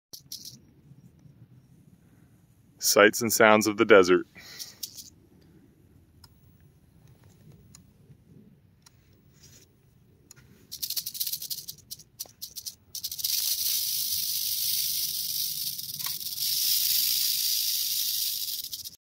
Hiking in the Desert find: Tiger Rattlesnake!
An early morning hike in the desert around Phoenix turned up this gorgeous Tiger Rattlesnake!